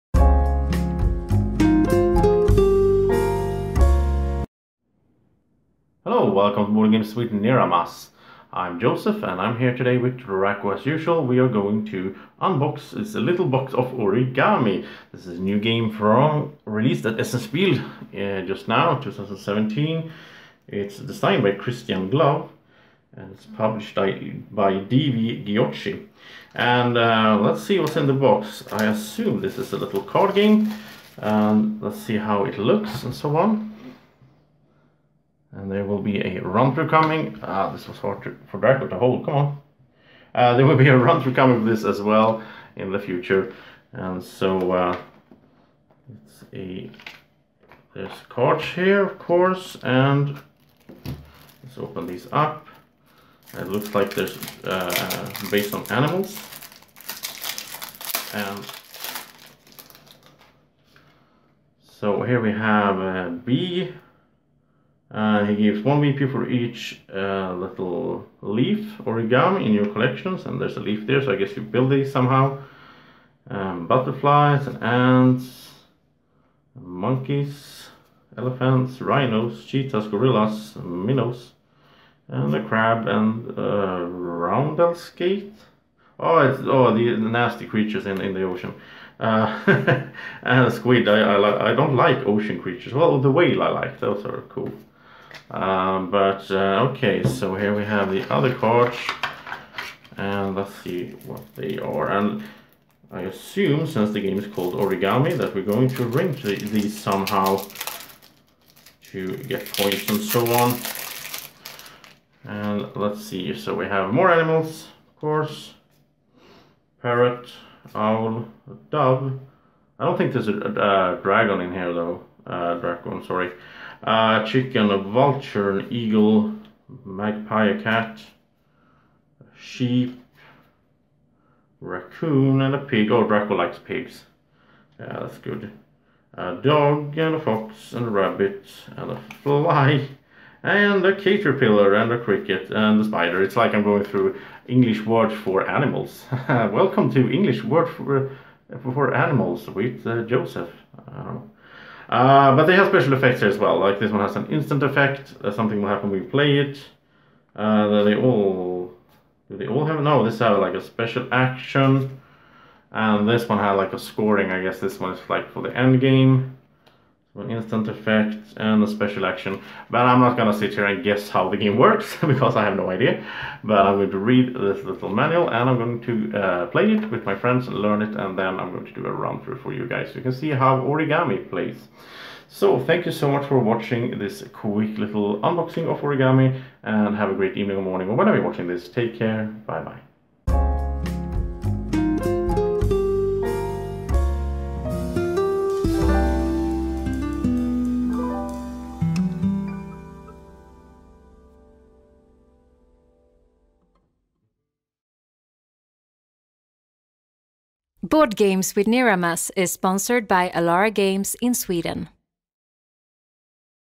Origami - Unboxing
Fresh out of Essen Spiel 2017, Origami is a new, small card game from dV Giochi. Niramas takes a look at the cards in the box while Drako learns english names for different animals!

Origami is designed by Christian Giove and published by dV Giochi in 2017.

Like and follow the Facebook page for bgwNiramas !